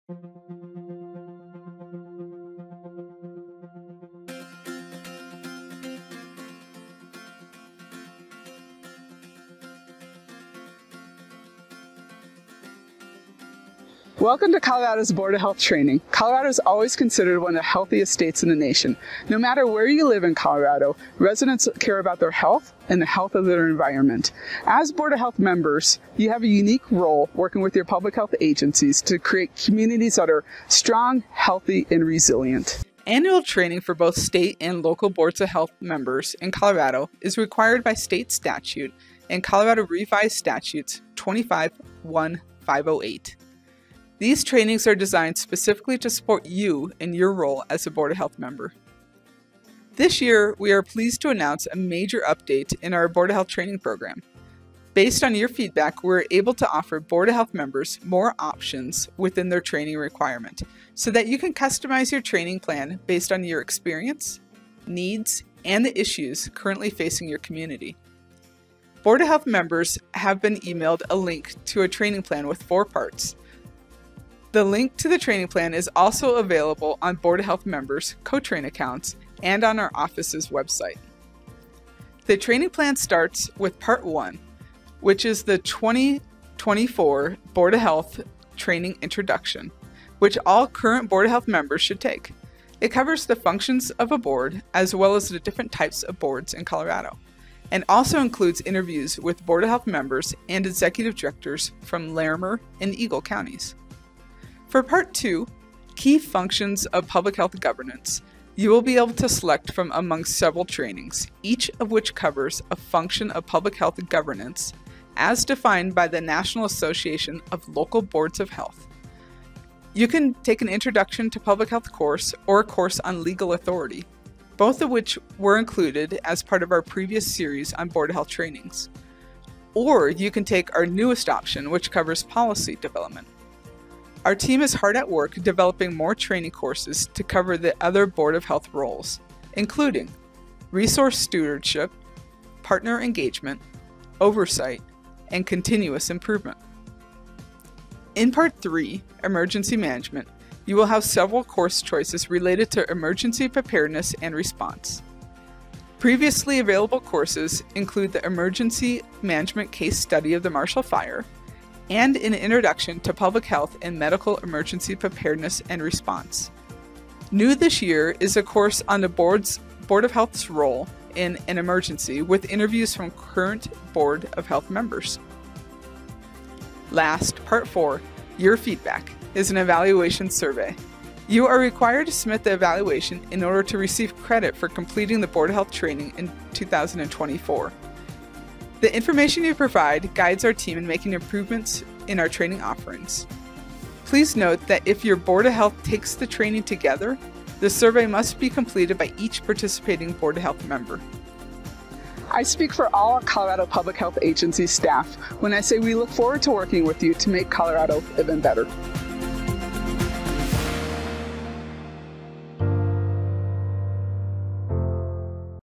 Colorado Board of Health Members Training 2024
This introduction to required training for board of health members in Colorado includes an overview of the new options available for board of health members.